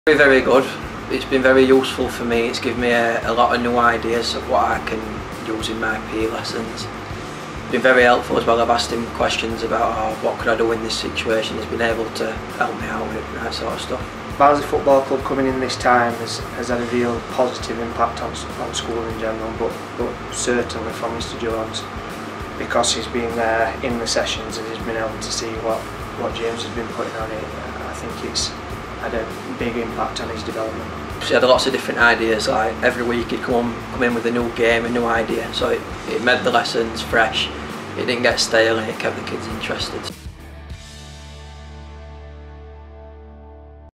Premier League Primary Stars | Tom's Story
See how the Premier League Primary Stars programme has helped Lacewood Primary School's Sports and Play Assistant Tom Jones.